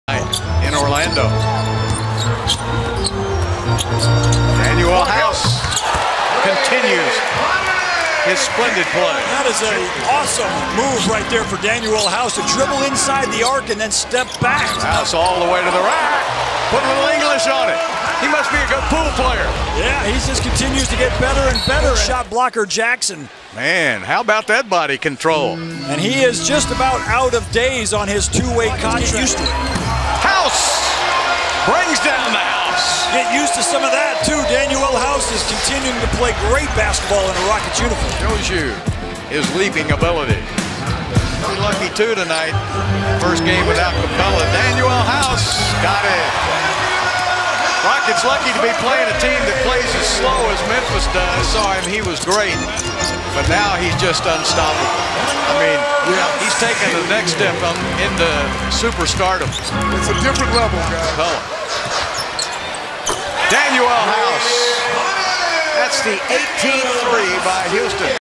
Danuel House 15 Points Full Highlights (1/14/2019)
Lost in the glorious, overpoweringly bright aura of James Harden's 57-point game is the smaller, less luminary aura of Danuel House's 15-point game. He was the second-best scorer on the team, and while his contributions didn't directly contribute to an actual win (because the Rockets won by more points [18] than House scored [15]), they did contribute to in-arena excitement as well as to my substantial (and growing) selection of Danuel House highlight videos. Those are significant contributions, in my opinion.

However, just as nobody remembers how Smush Parker scored 13 at the same time Kobe scored 81, or how J.R. Smith scored 14 when Carmelo scored 62 (that's the dude your team waived, Rockets fans), nobody is going to remember the heroic efforts of Danuel House to continue scoring the rock even when it was obvious that the only person scoring the rock should have been Harden. The aura of his performance is, if we're being honest, completely invisible when held against the heavenly glow of Harden's game, but it's still there if you take it to the side and look at it by itself. Such is the curse of the role-player.

I will say that, if I find out that House didn't invite Harden and the rest of the Rockets to a "House Party" after the game, I'm going to be quite upset.